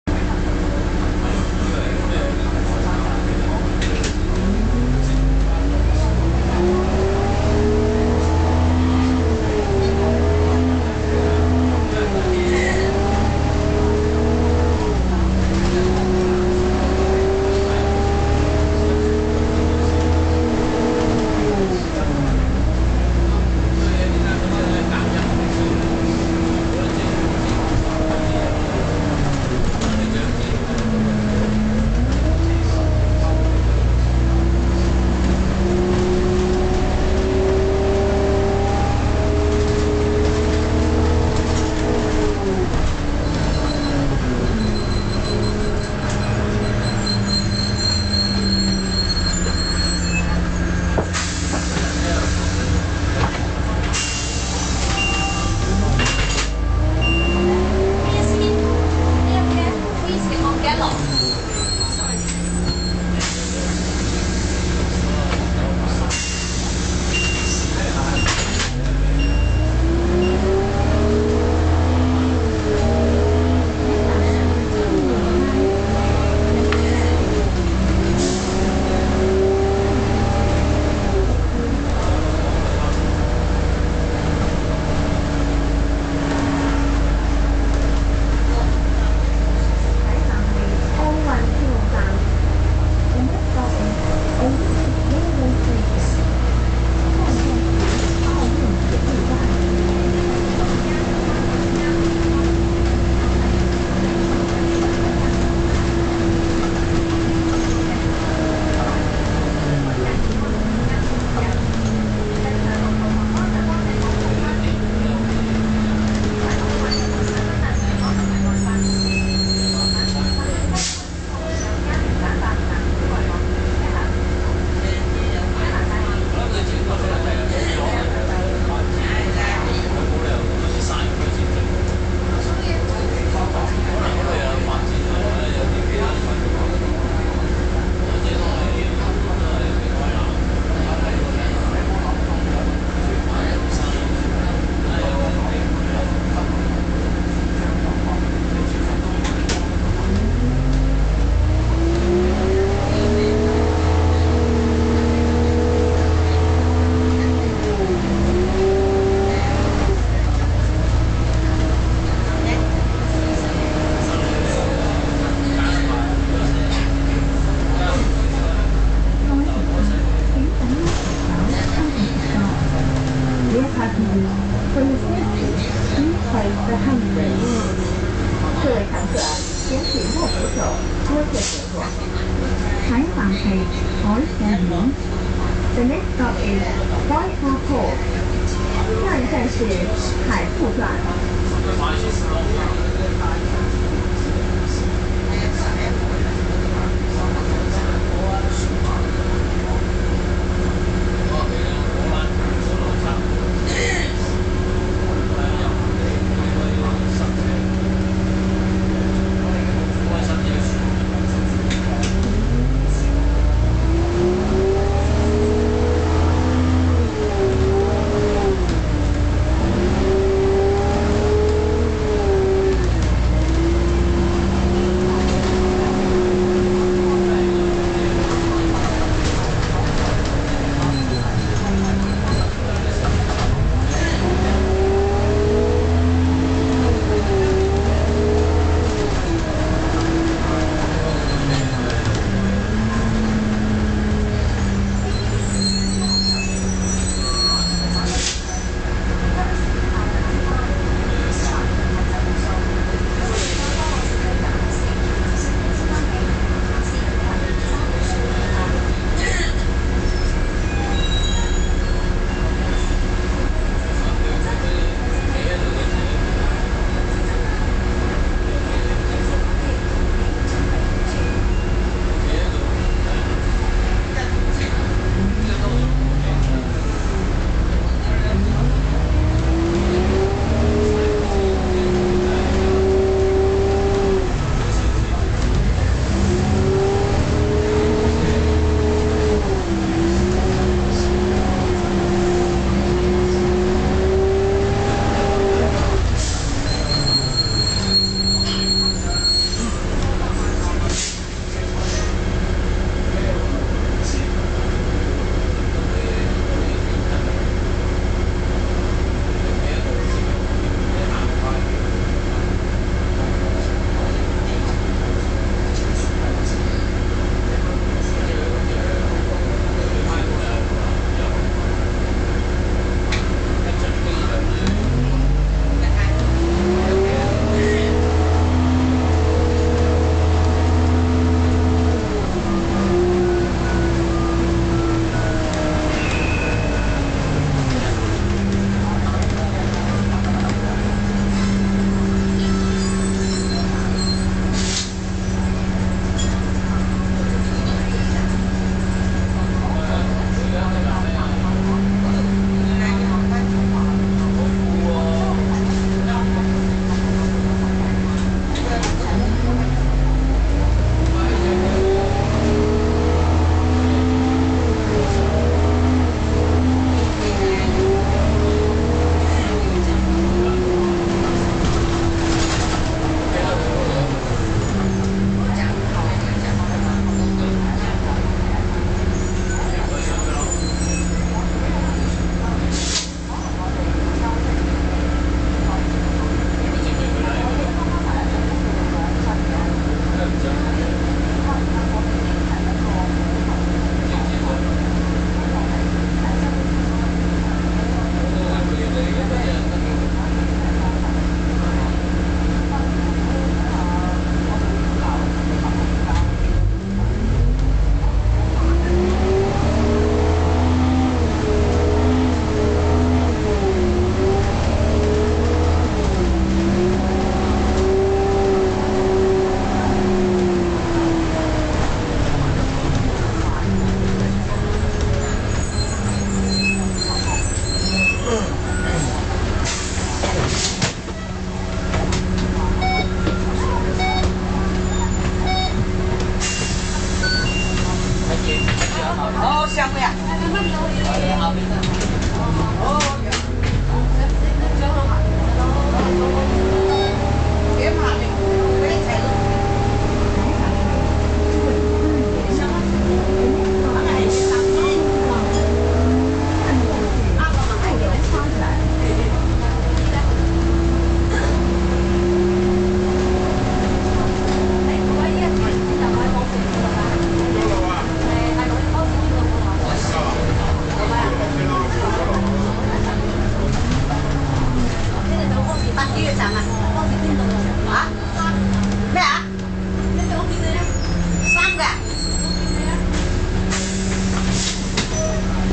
KMB Leyland Olympian 11M - AL109 FE8498 on 12
From Fu Chong Estate to Charming Garden